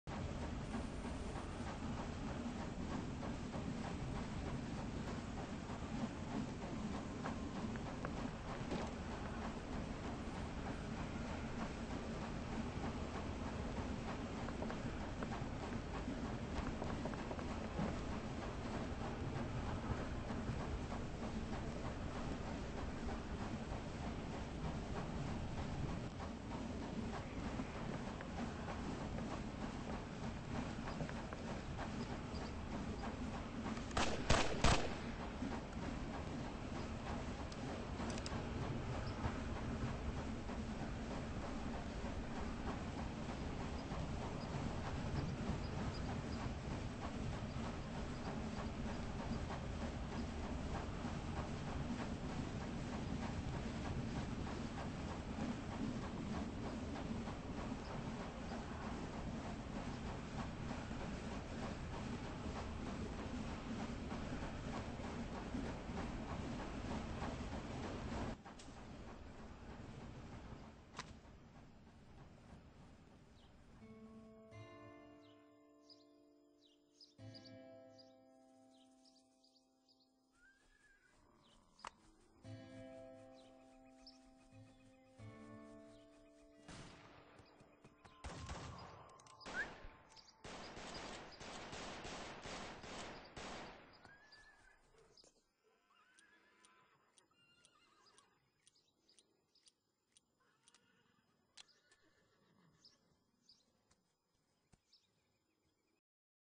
Glitch
Teleporting glitch on the train. WARNING does not teleport far.